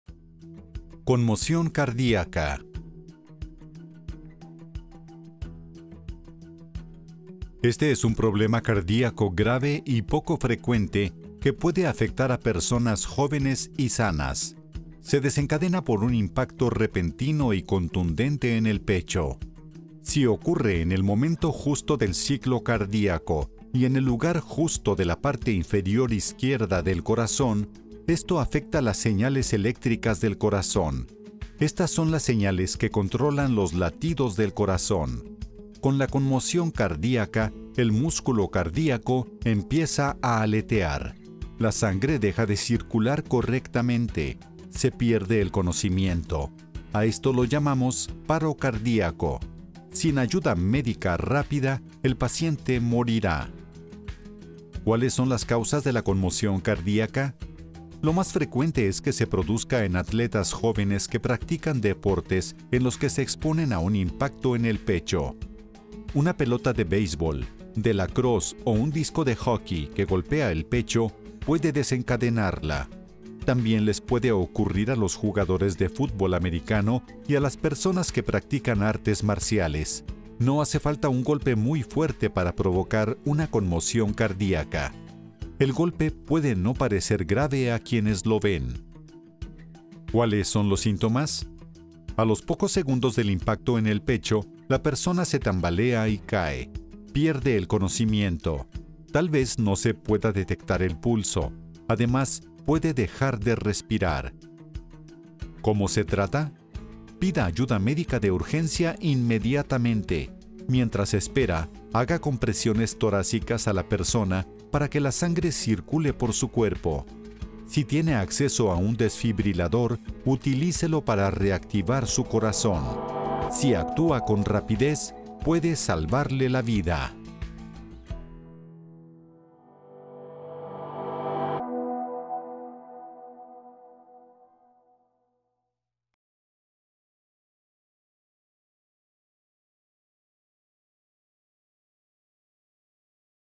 Conmoción cardíaca (Animación 3D)
Los deportistas que practican deportes que los ponen en riesgo de recibir un impacto en el pecho, como el fútbol o las artes marciales, corren el riesgo de sufrir commotio cordis. Se desencadena por un impacto repentino y contundente en el pecho y puede provocar un paro cardíaco y la muerte.

Para obtener la licencia de este video y miles más, visite